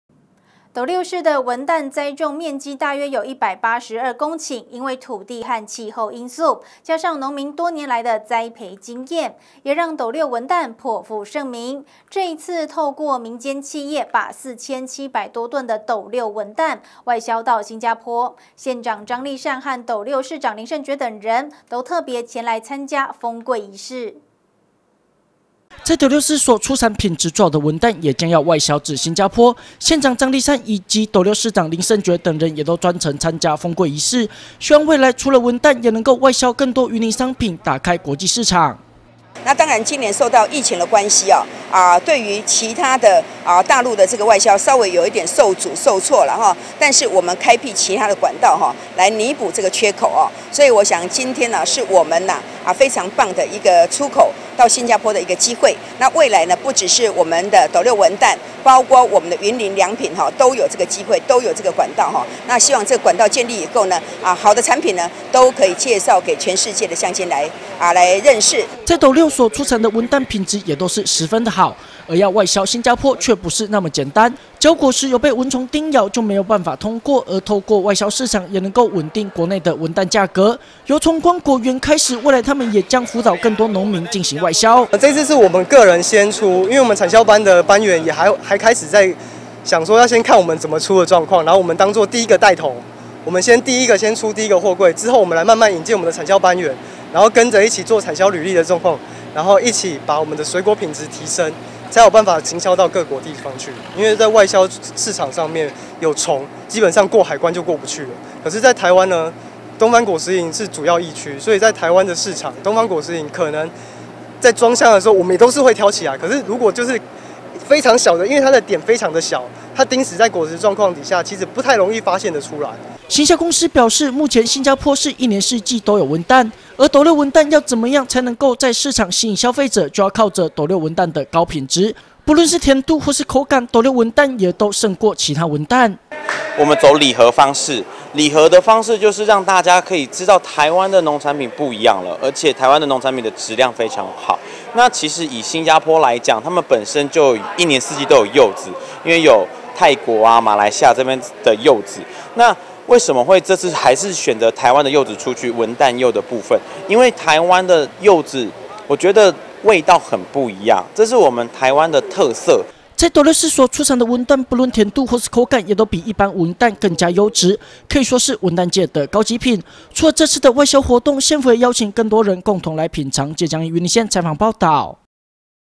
中台灣生活網 雲林縣長出席斗六文旦封櫃儀式
斗六市的文旦栽種面積大約有182公頃，因為土地以及氣候因素，加上農民多年來的栽培經驗，也讓斗六文旦頗富盛名，而這次也來透過民間企業，把4700多噸的斗六文旦，外銷到新加坡，縣長張麗善以及斗六市長林聖爵等人，都特別前來參加封櫃儀式。